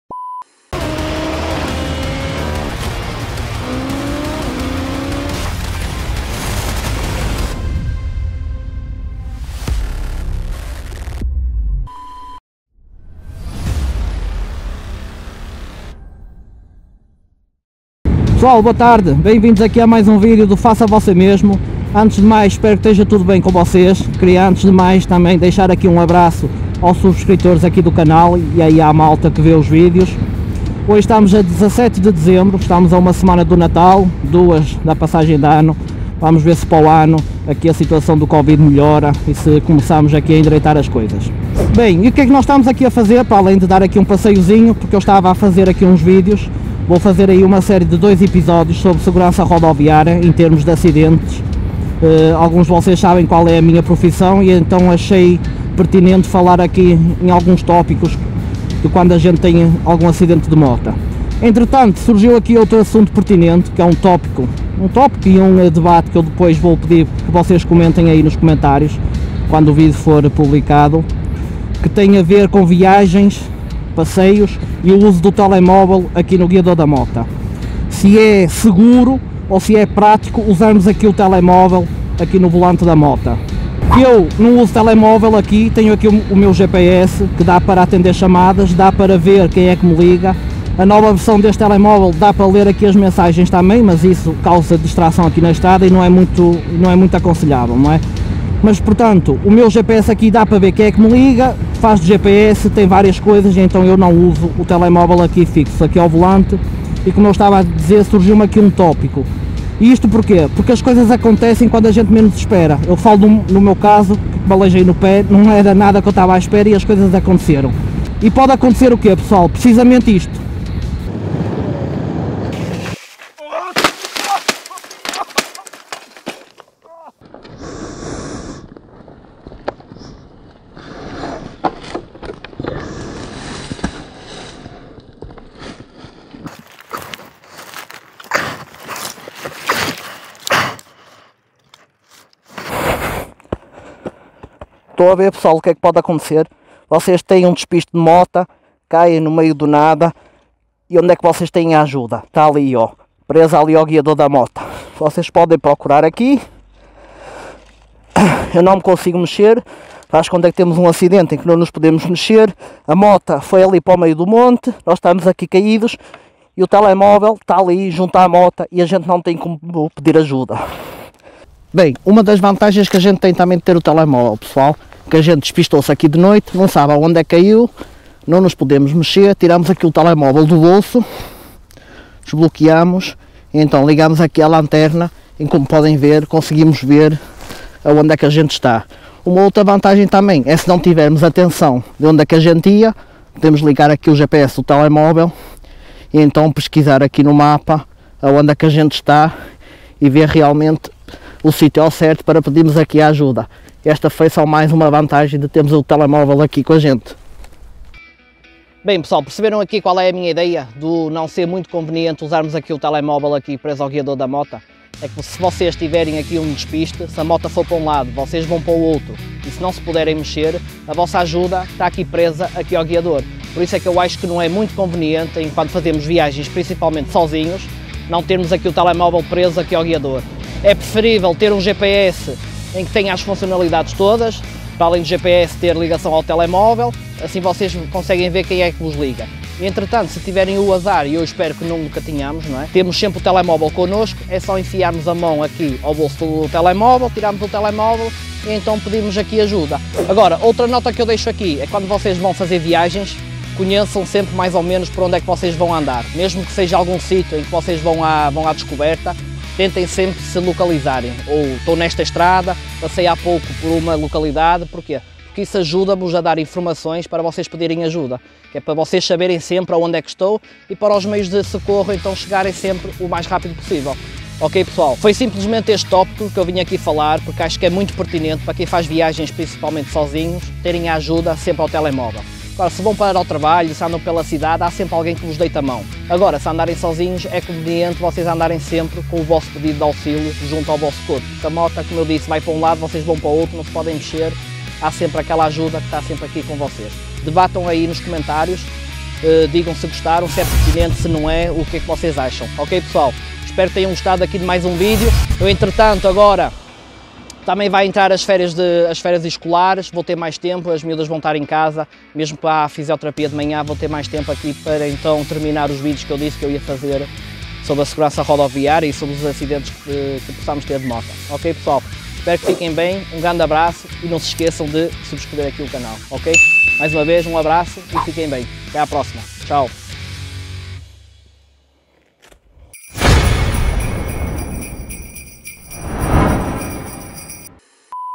Será SEGURO & PRÁTICO uso telemóvel fixo no guiador da mota em caso de ACIDENTE
Já se imaginaram numa viagem sozinhos? No meio do nada, de noite ou de dia... E o azar aparece-vos numa curva?
E se ficarem imobilizados no chão, o telemóvel na mota, etc... Como pedem auxílio??
Quantos de vocês já se lembraram do assunto?